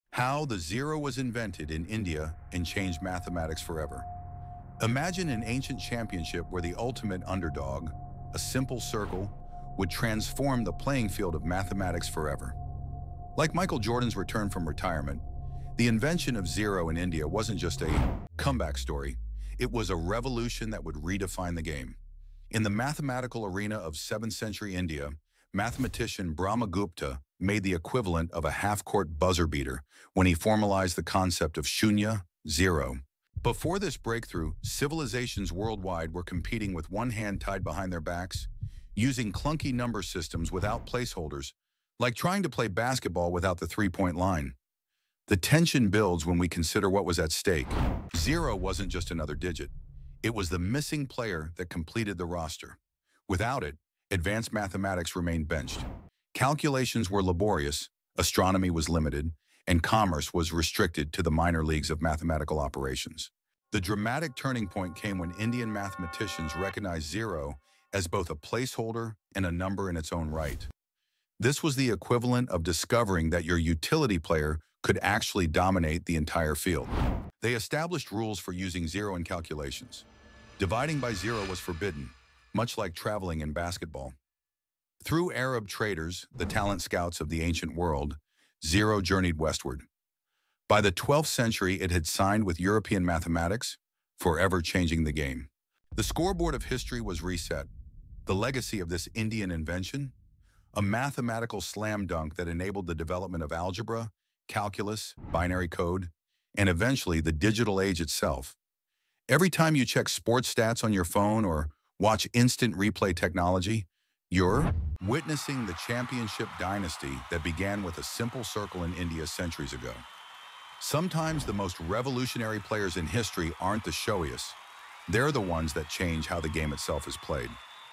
India's ZERO Revolutionized Mathematics Forever
Imagine an ancient championship where the ultimate underdog-a simple circle-would transform the playing field of mathematics forever. Like Michael Jordan's return from retirement, the invention of zero in India wasn't just a comeback story; it was a revolution that would redefine the game.